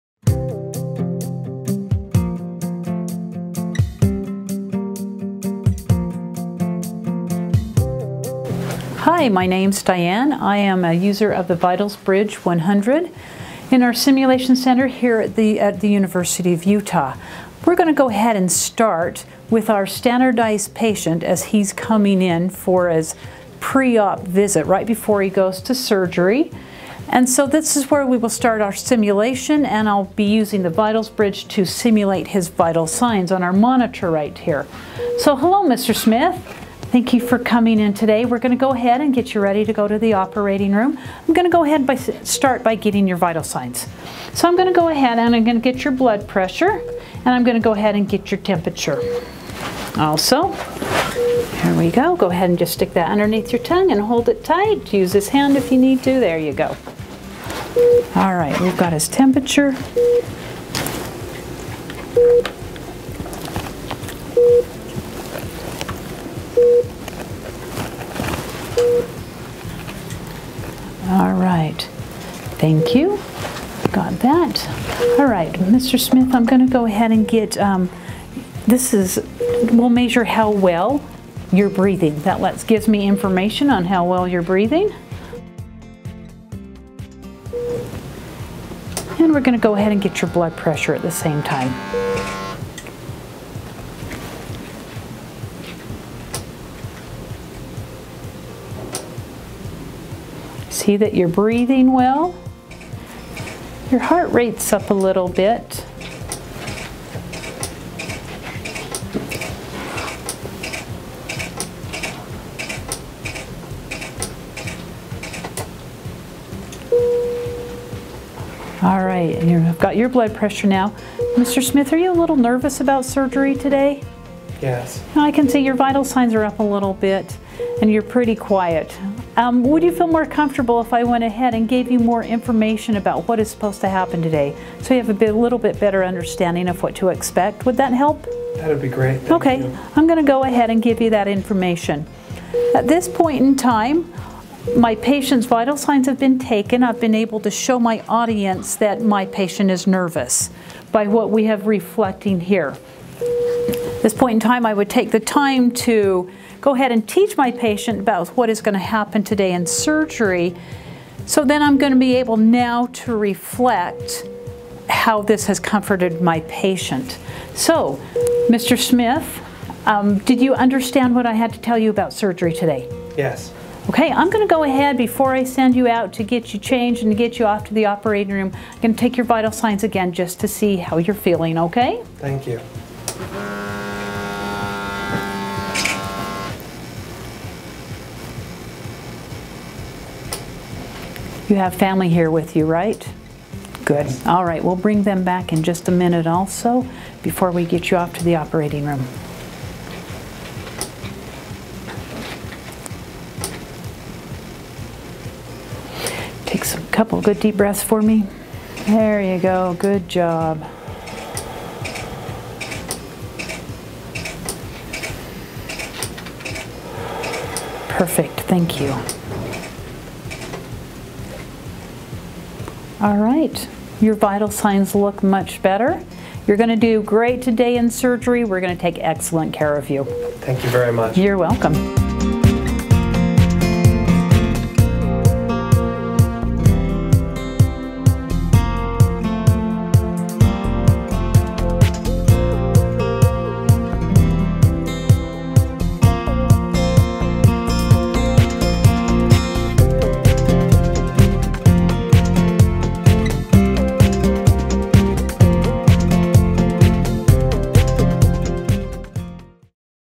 VitalsBridge 100 Standardized Patient and Real Patient Monitor Demo
Here is a demo of using the VitalsBridge 100 with a standardized patient and a real patient monitor displaying vital signs that match the scenario being portrayed